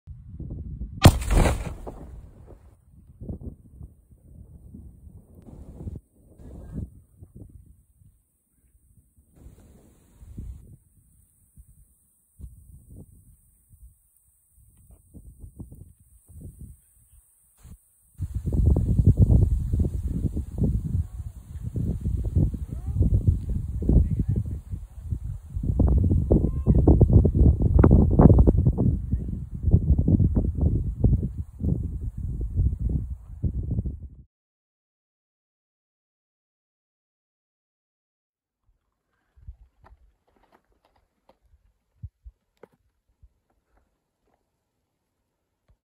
February 24, 2021: Explosive triggered persistent slab at Irwin
One of numerous persistent slabs triggered over the past few days on Irwin’s Upper Upper Westwall. West and southwest facing terrain